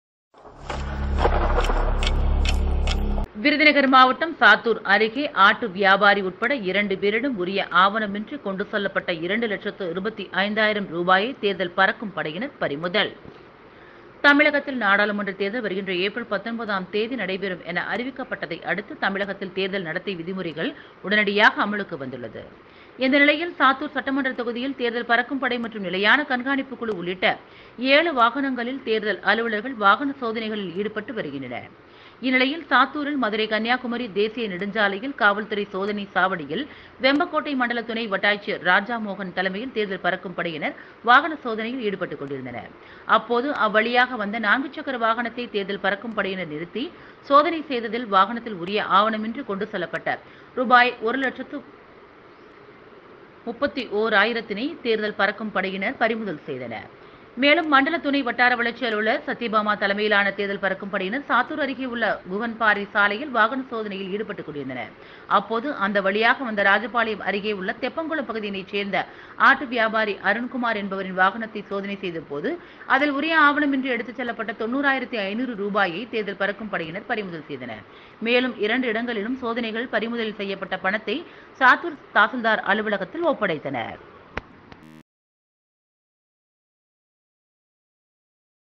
அருகே உரிய ஆவணமின்றி எடுத்து செல்லப்பட்ட இருவரது பணம் பறிமுதல்
cithiraitv சித்திரைடிவி விருதுநகர் மாவட்டம் சாத்தூர் அருகே ஆட்டு வியாபாரி உட்பட 2 பேரிடம் உரிய ஆவணம் இன்றி கொண்டு செல்லப்பட்ட 2 லட்சத்து 25.000 ரூபாயை தேர்தல் பறக்கும் படையினர் பறிமுதல்...

தமிழகத்தில் நாடாளுமன்றத் தேர்தல் வருகிற ஏப்ரல் 19 ம் தேதி நடைபெறும் அறிவிக்கப்பட்டதை அடுத்து தமிழகத்தில் தேர்தல் நடத்தை விதிமுறைகள் உடனடியாக அமுலுக்கு வந்து உள்ளது.

இந்த நிலையில் சாத்தூர் சட்டமன்றத் தொகுதியில் தேர்தல் பறக்கும் படை மற்றும் நிலையான கண்காணிப்பு குழு உள்ளிட்ட 7 வாகனங்களில் தேர்தல் அலுவலர்கள் வாகன சோதனைகளில் ஈடுபட்டு வருகின்றனர்

இந்த நிலையில்  சாத்தூரில்  மதுரை கன்னியாகுமரி தேசிய நெடுஞ் சாலையில் காவல் துறை சோதனை சாவடியில்  வெம்பக்கோட்டை  மண்டல துணை வட்டாட்சியர் ராஜா மோகன் தலைமையில் தேர்தல் பறக்கும் படையினர் வாகன சோதனையில் ஈடுபட்டுக் கொண்டிருந்தனர்.

 அப்போது அவ்வழியாக வந்த நான்கு சக்கர வாகனத்தை தேர்தல் பறக்கும் படையினர் நிறுத்தி சோதனை செய்ததில் வாகனத்தில் உரிய ஆவணமின்றி கொண்டு செல்லப் பட்ட ரூபாய் 1.31லட்சம் பணத்தை தேர்தல் பறக்கும் படையினர் பறிமுதல் செய்தனர்.

மேலும்  மண்டல துணை வட்டார வளர்ச்சி அலுவலர் சத்யபாமா தலைமையிலான தேர்தல் பறக்கும் படையினர் சாத்தூர் அருகே உள்ள குகன் பாறை சாலையில் வாகன சோதனையில் ஈடுபட்டுக் கொண்டிருந்தனர்.

அப்போது அந்த வழியாக வந்த ராஜபாளையம் அருகே உள்ள  தொம்பகுளம் பகுதியை சேர்ந்த ஆட்டு வியாபாரி அருண்குமார் (29) என்பவரின் வாகனத்தை சோதனை செய்த போது அதில் உரிய ஆவண மின்றி எடுத்துச் செல்லப்பட்ட 90,500 ரூபாயை தேர்தல் பறக்கும் படையினர் பறிமுதல் செய்தனர்.

மேலும் இரண்டு இடங்களில் சோதனைகள் பறிமுதல் செய்யப்பட்ட பணத்தை சாத்தூர் தாசில்தார் அலுவலகத்தில் ஒப்படைத்தனர்